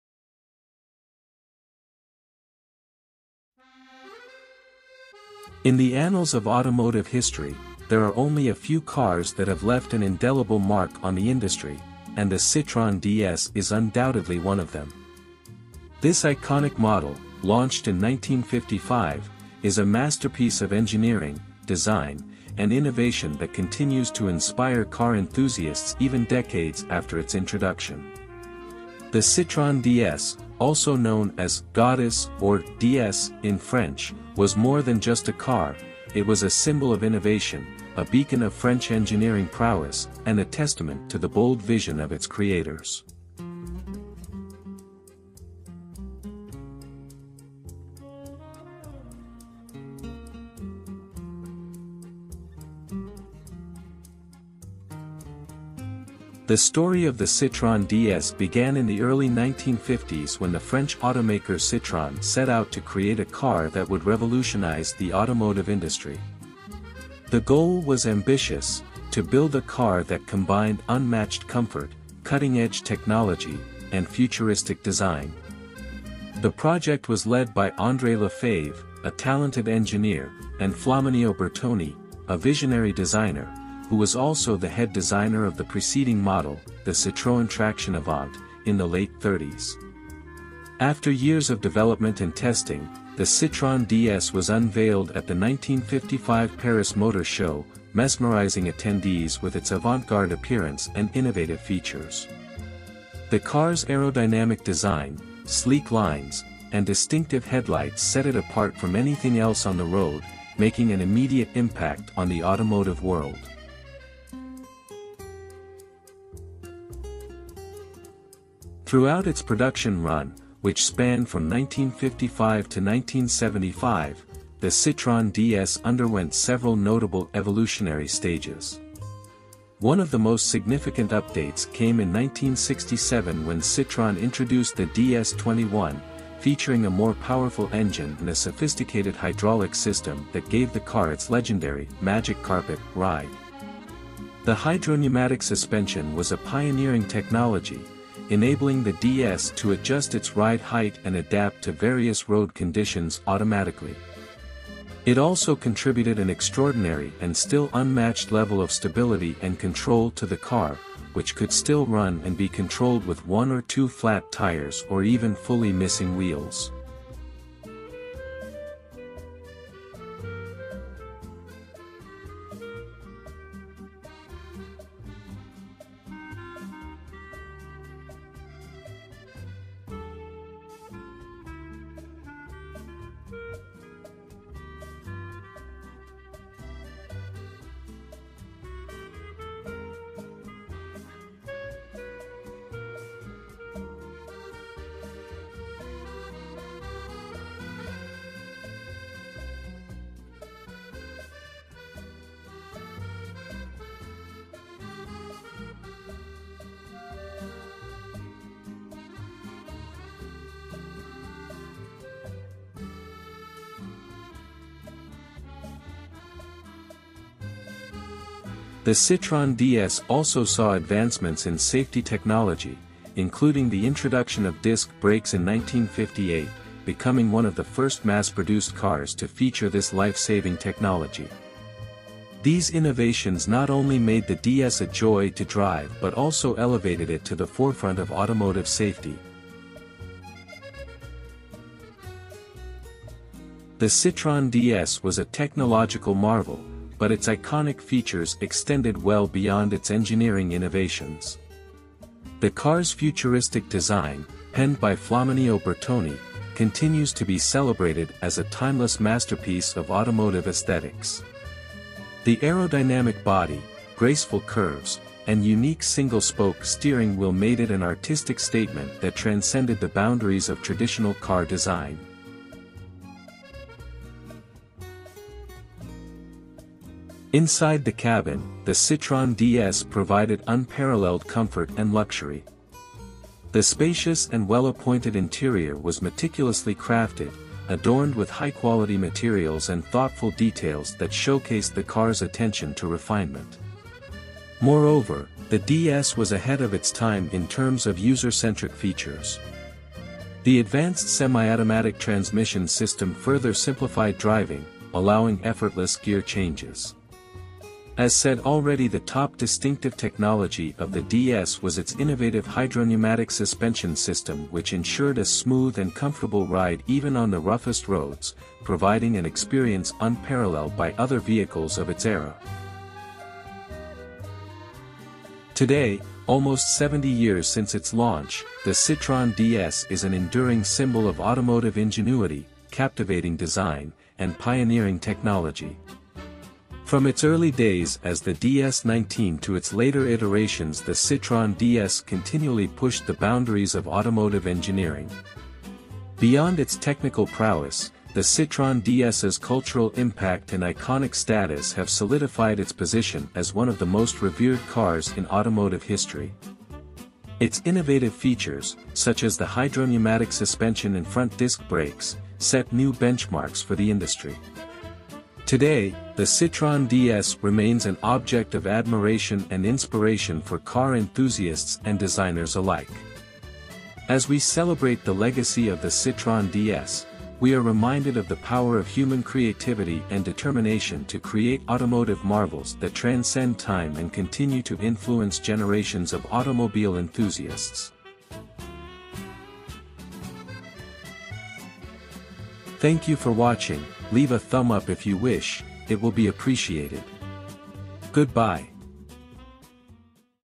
The Citroën DS: An Icon of Automotive Brilliance
Launched in 1955, the Citroen DS was a marvel of engineering, design, and forward-thinking technology that redefined the very essence of what a car could be. Even today, it remains an enduring testament to the unparalleled innovation that can shape the automotive world.